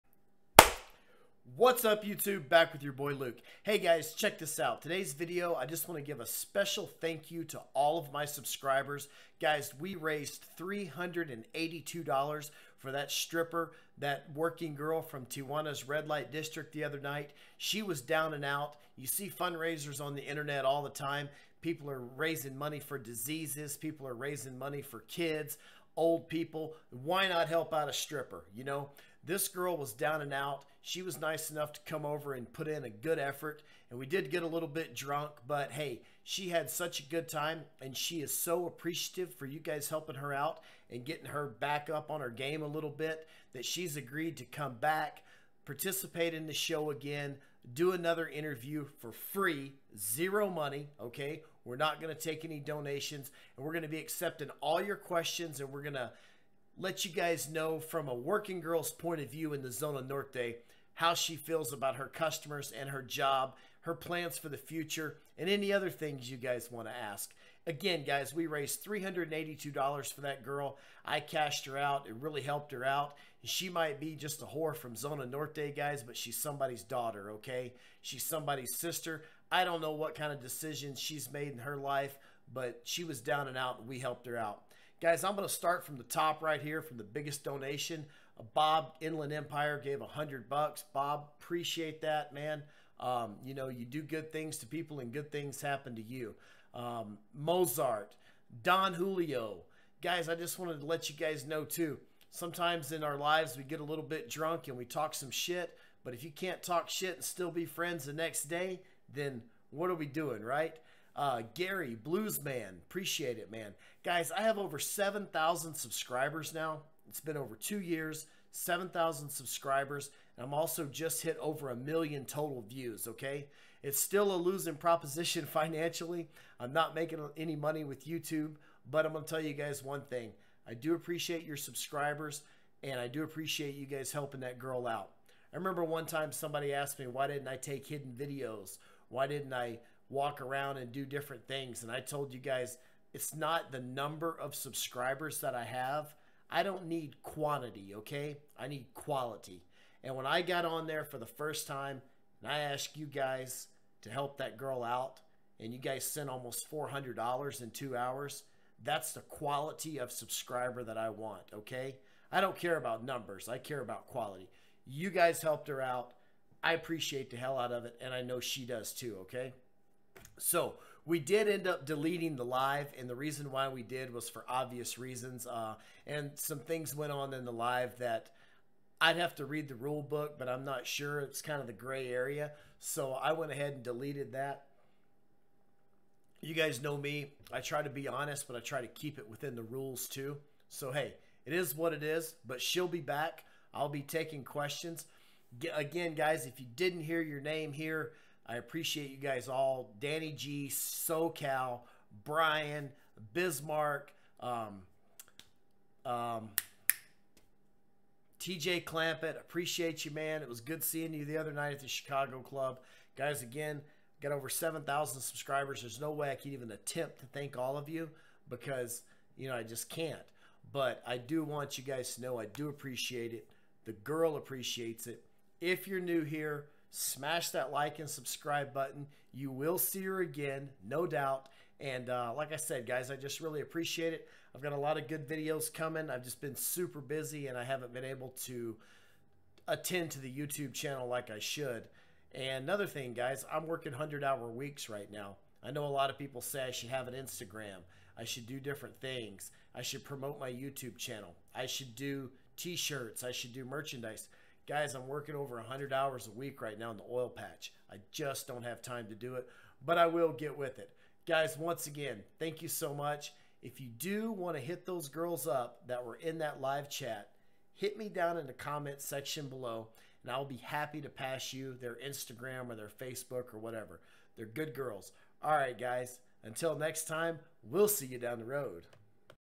Interview with Zona Norte Tijuana working girls and 1 million views! (Thanks)
Chanel updates